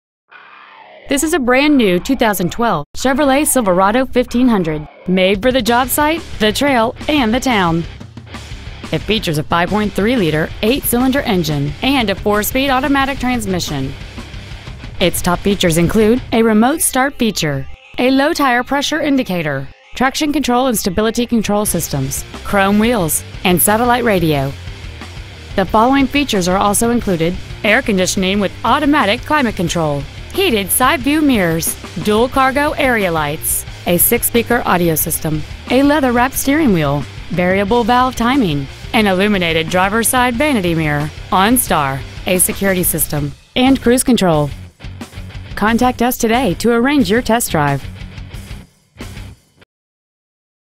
2012 Chevrolet Silverado 1500 Gainesville GA 30501
Year : 2012
 Make : Chevrolet
 Model : Silverado 1500
 Engine : Gas/Ethanol V8 5.3L/323
 Trans . : Automatic
 Exterior : Black Granite Metallic

 Interior : 19C EBONY
 HARDY CHEVROLET INC
 2115 Browns Bridge Road
 Gainesville , GA 30501
 2012 Chevrolet Silverado 1500 Gainesville GA